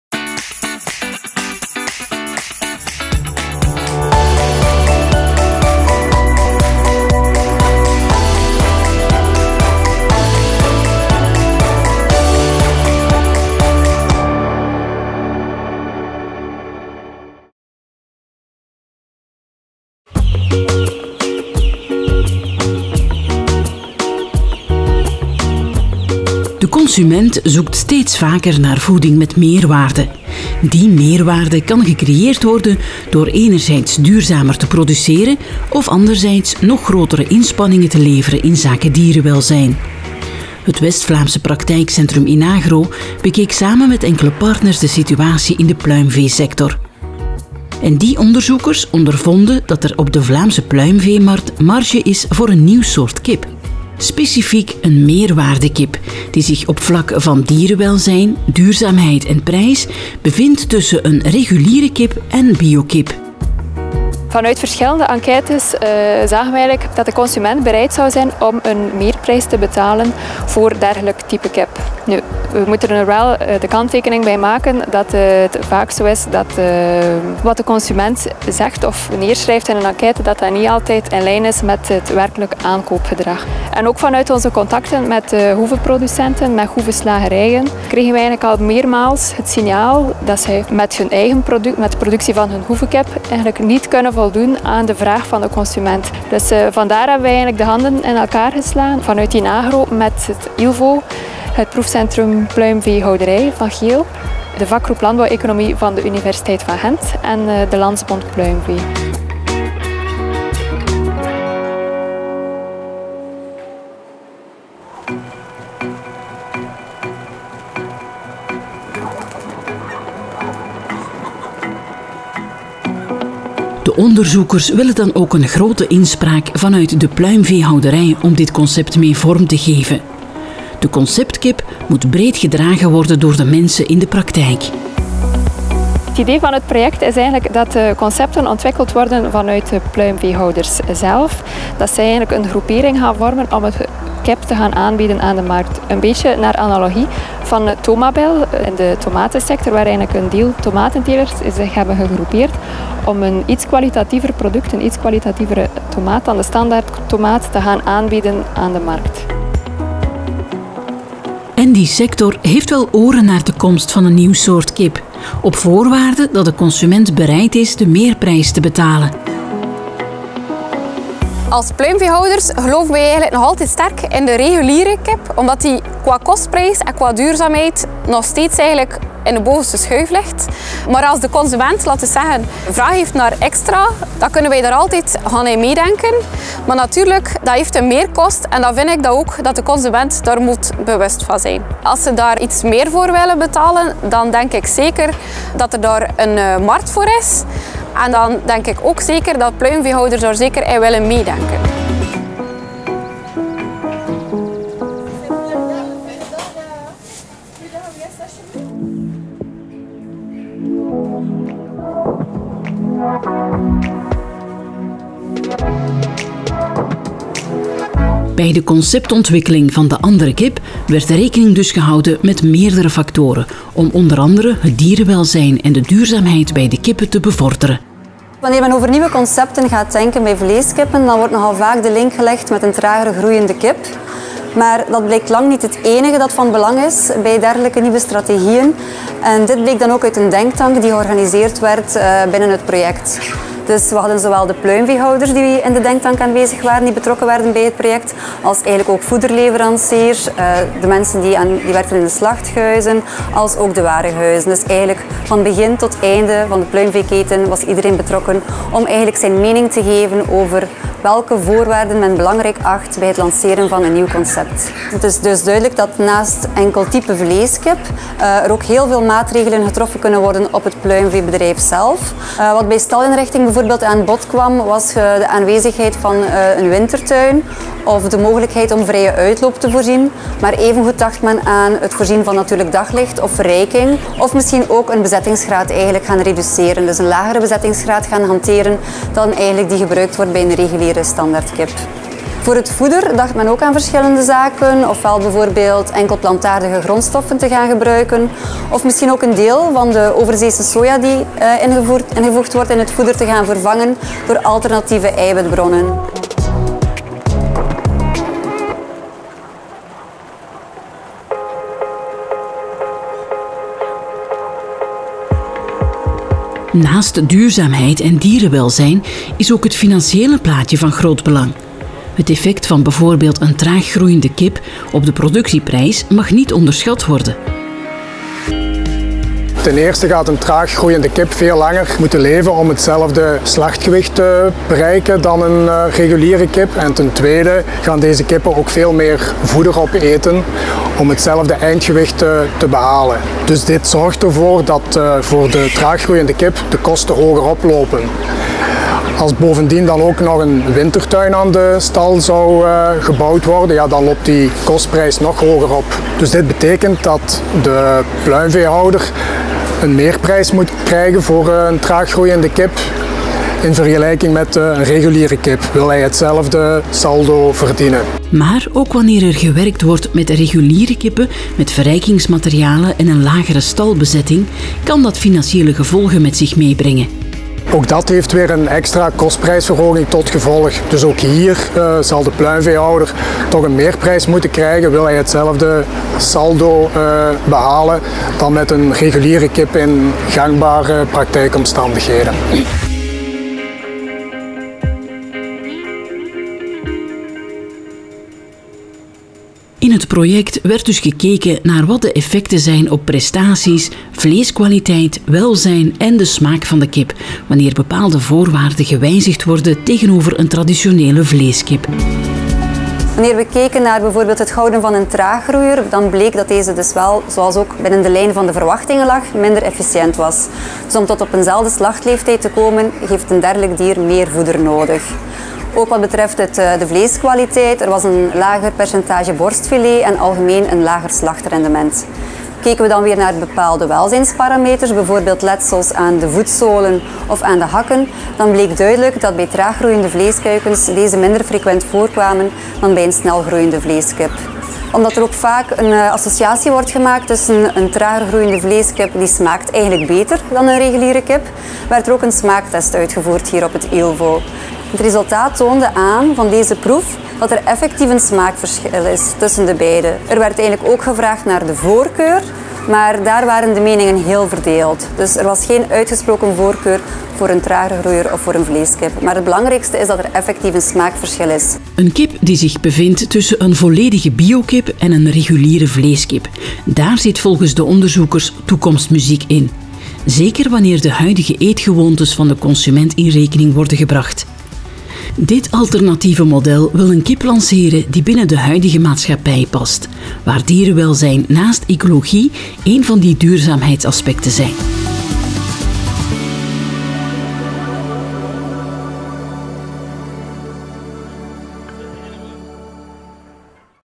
Tournée Provinciale - De Andere Kip
Is er op de pluimveemarkt ruimte voor een nieuw soort kip, een kip die zich op het vlak van dierenwelzijn, duurzaamheid én prijs tussen een reguliere kip en een biokip bevindt? Samen met enkele partners deden we onderzoek via het project 'De Andere Kip'.